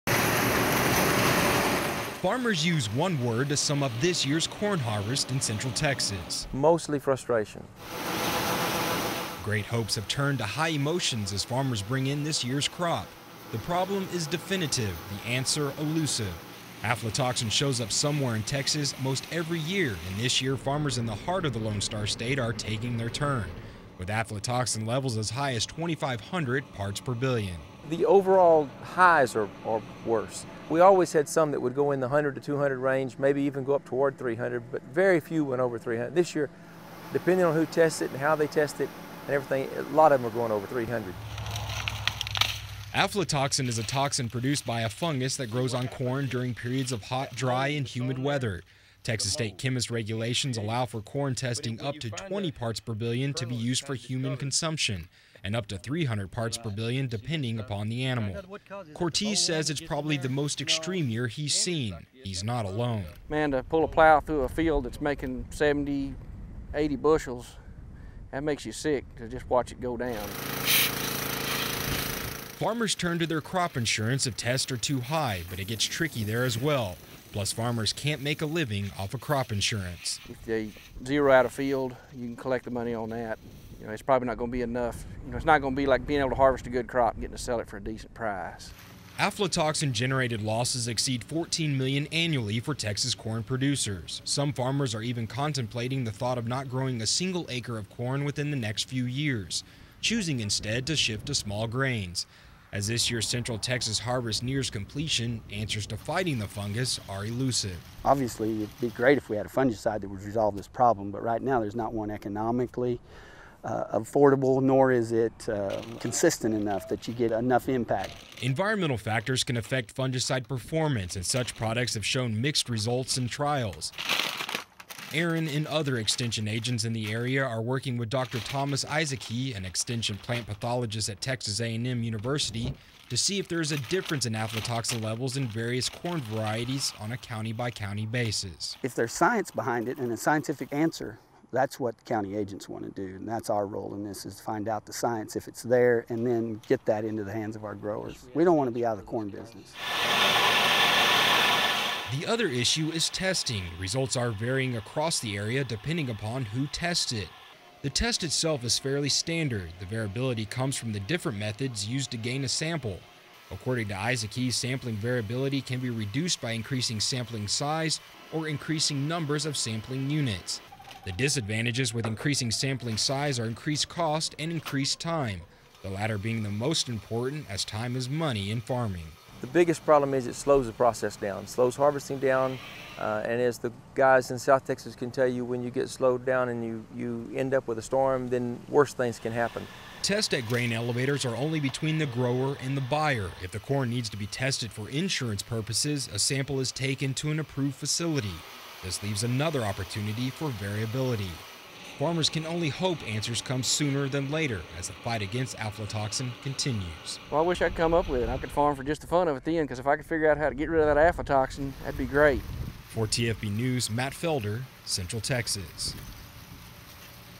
A Search for Answers - Aflatoxin
What would cause a farmer to plow up a seemingly productive corn crop?  The culprit is a fungus called Aflatoxin and its proving to be devastating to Central Texas farmers.  Matt Felder reports.